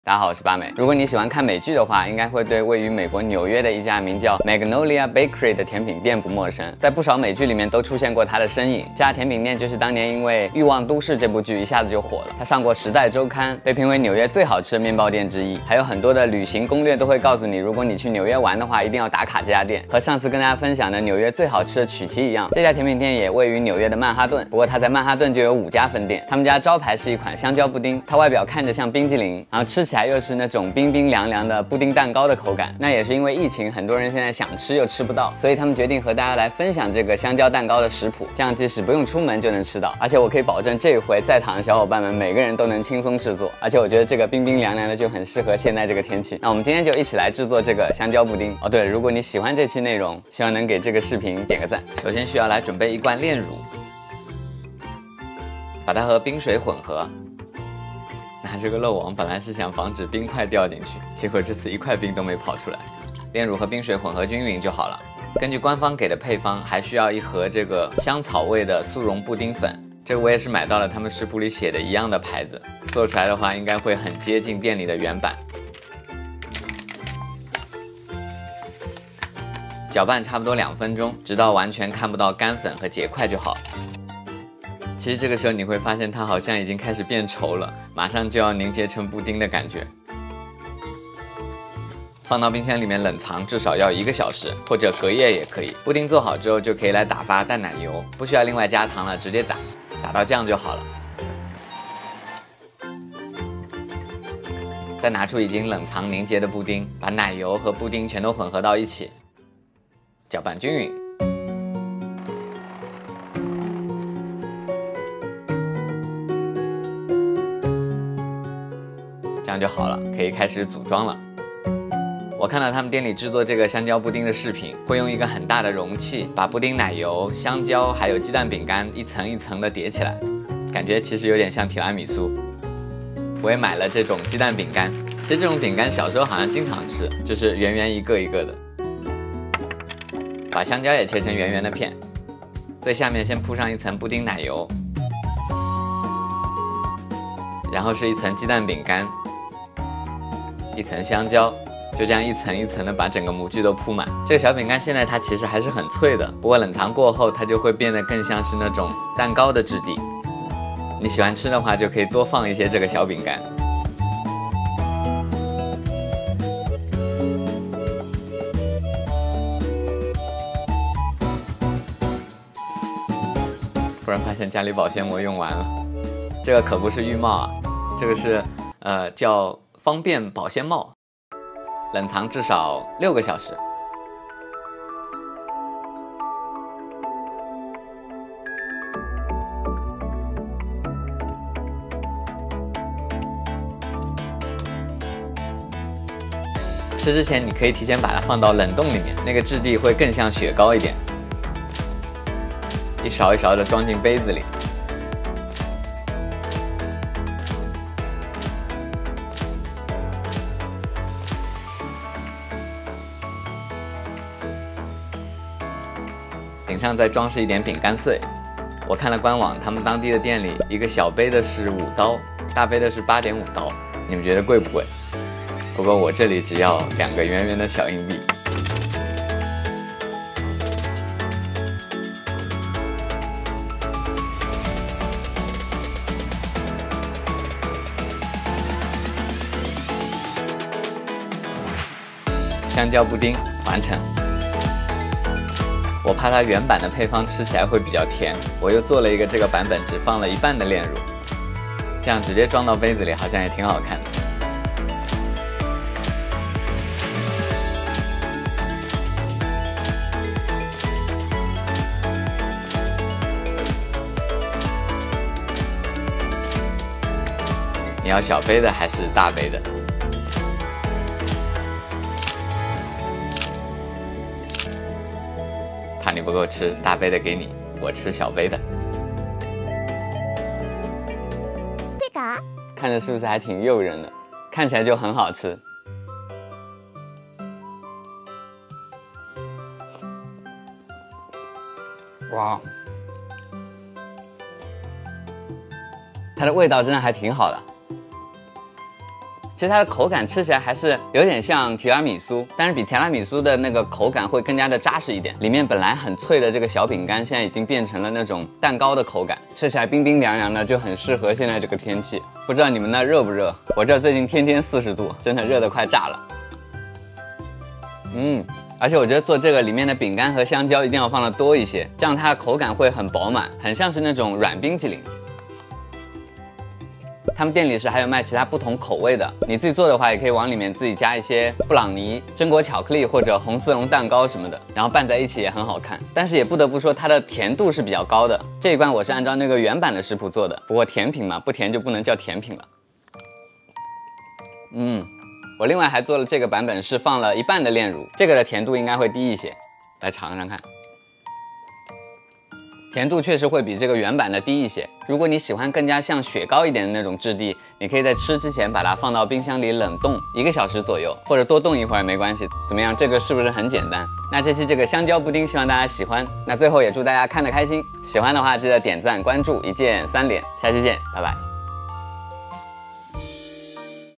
香蕉布丁🍮 | 這個冰淇淋布丁無敵了！紐約街頭最好吃的香蕉布丁！【Banana Pudding】| 元气八眉菌
這個冰淇淋布丁無敵了！紐約街頭最好吃的香蕉布丁！【Banana Pudding】
炎熱的夏天，一起動手來做甜品吧～這期香蕉布丁做起來真的超簡單！ ！很適合夏天和家人朋友一起分享，大家一定要收藏起來試試看！ ！詳細食譜和做法在視頻最後～

喜歡我的視頻，請按贊、分享哦~